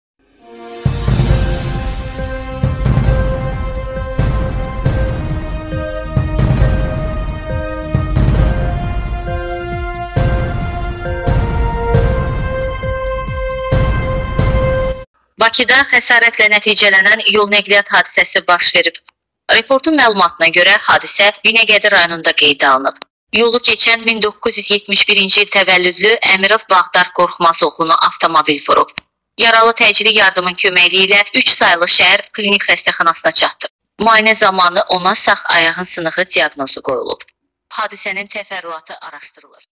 Baki və Binəqədidə yol qəzası xəsarət alan var
Binəqədidə yol qəzası  xəsarət alan var

Binəqədi rayonunda avtomobil piyadanı vurdu

Bakıda xəsarətlə nəticələnən yol-nəqliyyat hadisəsi baş verib.

"Report"un məlumatına görə, hadisə Binəqədi rayonunda qeydə alınıb. Yolu keçən 1971-ci il təvəllüdlü Əmirov Bağdar Qorxmaz oğlunu avtomobil vurub. Yaralı təcili yardımın köməkliyi ilə 3 saylı şəhər Klinik Xəstəxanasına çatdırılıb. Müayinə zamanı ona sağ ayağın sınığı diaqnozu qoyulub.

Hadisənin təfərrüatı araşdırılır.